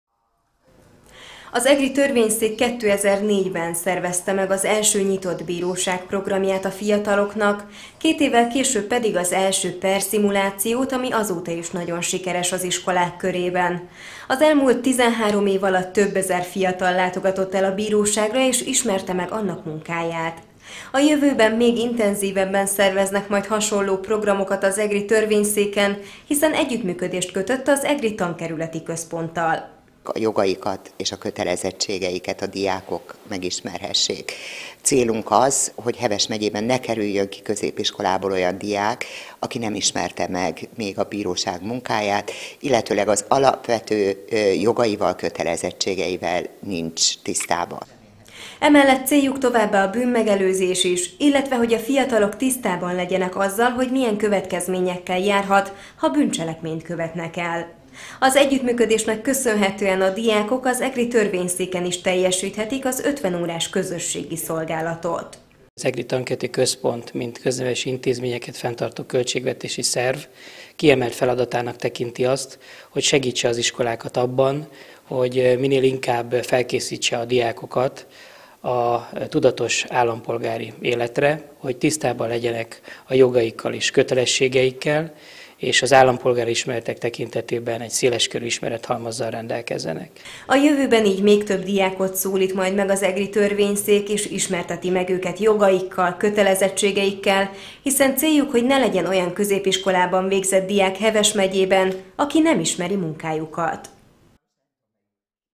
Megállapodás – Még több fiatal ismerkedhet meg a bíróság munkájával - 2017.09.13.
Együttműködési megállapodást kötött az Egri Törvényszék és az Egri Tankerületi Központ. Céljuk, hogy ne legyen olyan középiskolában végzett diák Heves megyében, aki nem ismeri a bíróság munkáját, illetve a jogaival, kötelezettségeivel nincs tisztában.